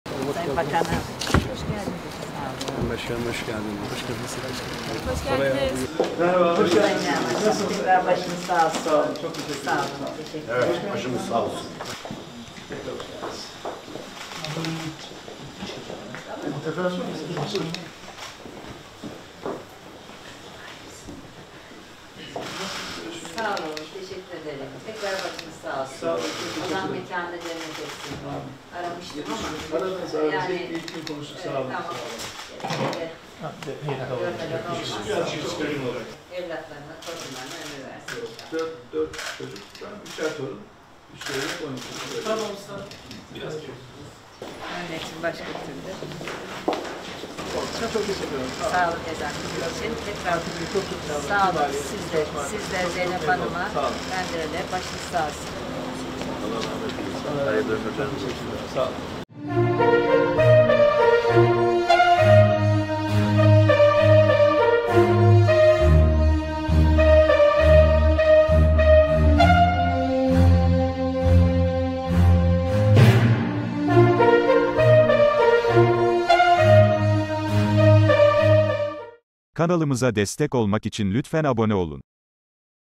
Meral Akşener'den, Ali Babacan'a taziye ziyareti
İYİ Parti Genel Başkanı Meral Akşener'den, geçtiğimiz hafta babasını kaybeden DEVA Partisi Genel Başkanı Ali Babacan'a taziye ziyareti